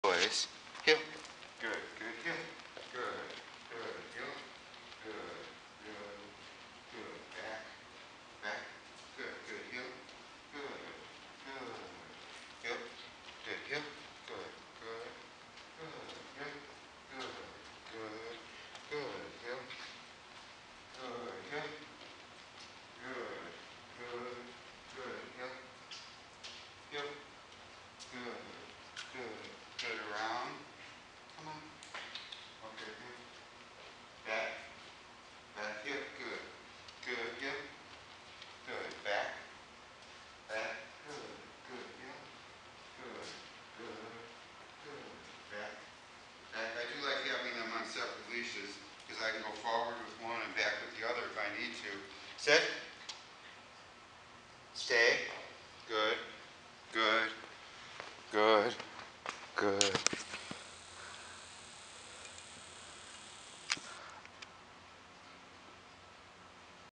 Allan Ross Dog Training
Winston and Bentley are West Highland Terriers I have worked with since they were 5 months old.  Here I practice with 2 dogs on my left in the Canine College.